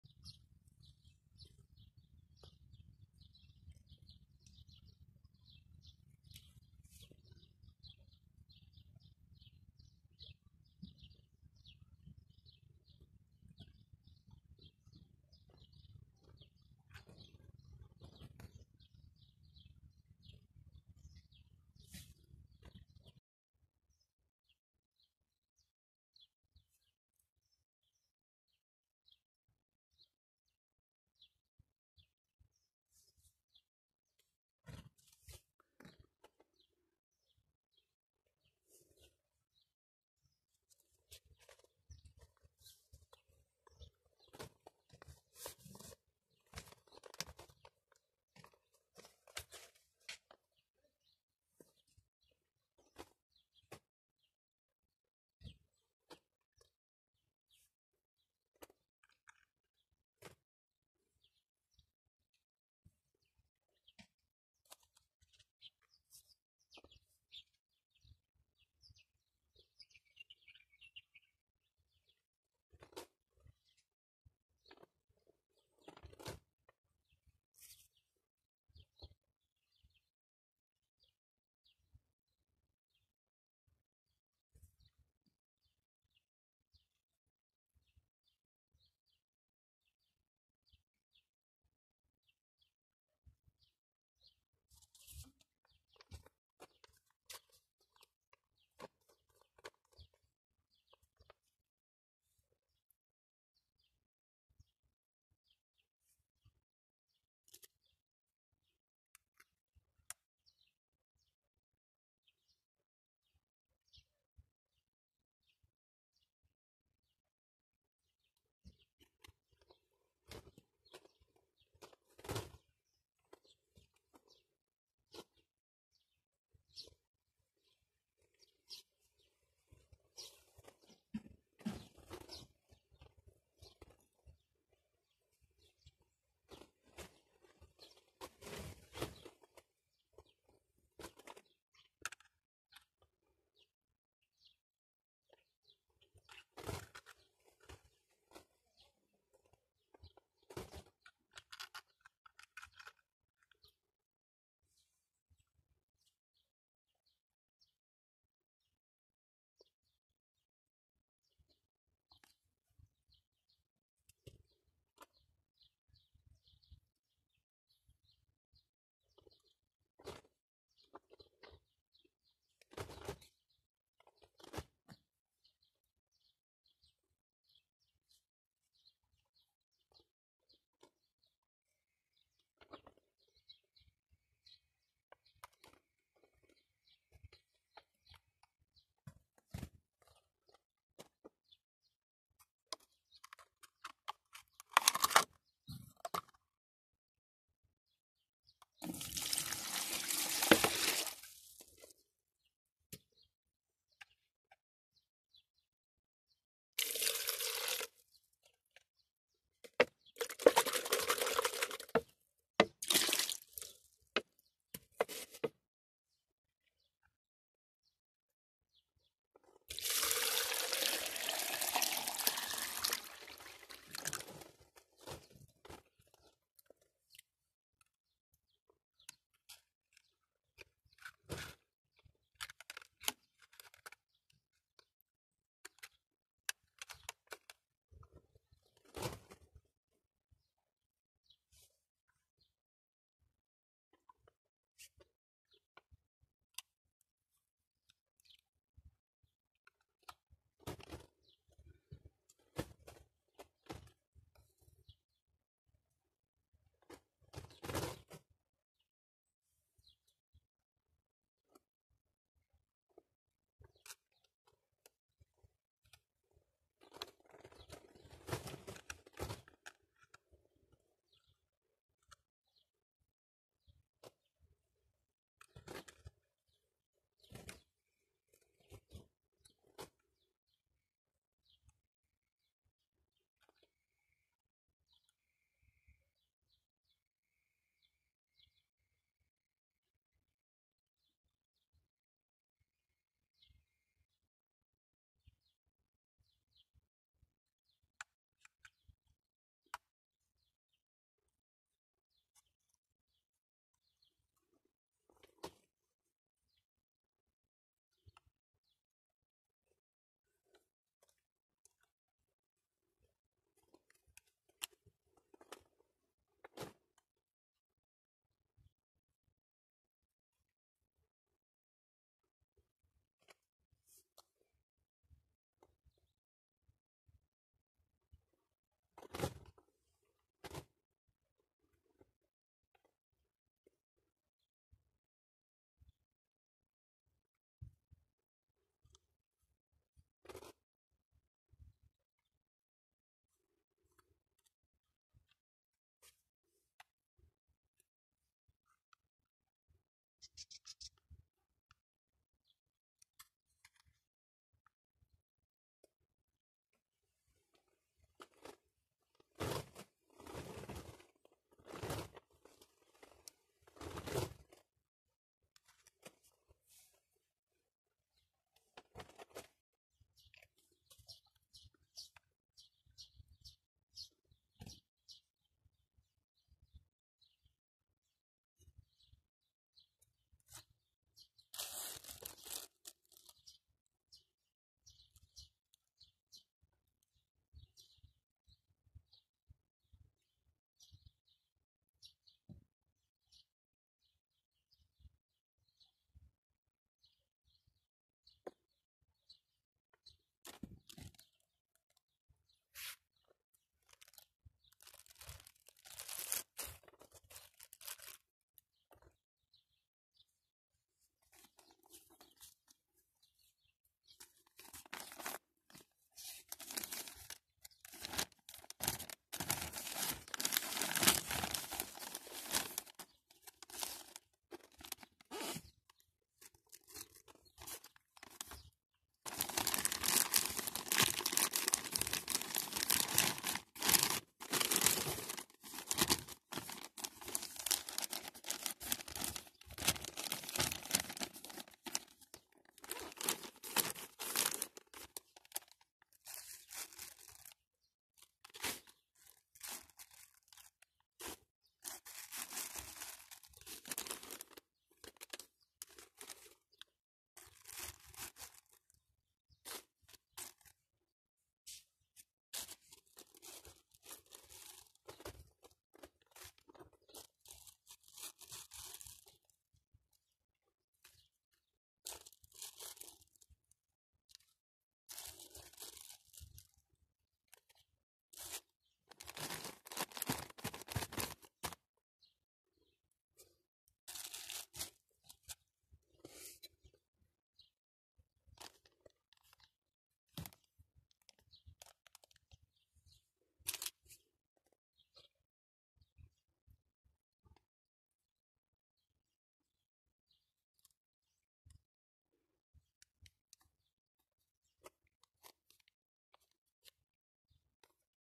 le secret de la réussite d'une avocatier à travers le noyau
dans cette vidéo il y a  des étapes à suivre pour obtenir une plante d'avocatier à travers le noyau, comment germer le noyau, et le résultat après la germination